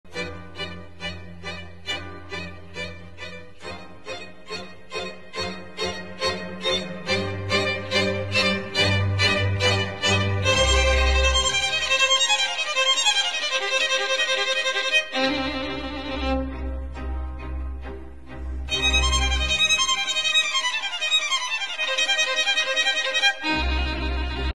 Ансамбль скрипачей «Кварта» ГрГМУ – участник конкурса «АРТ-портал-2024»
Ансамбль скрипачей «Кварта» студенческого клуба ГрГМУ участвует в конкурсе «АРТ-портал-2024», в номинации «Хобби-версии:АРТ-пиар».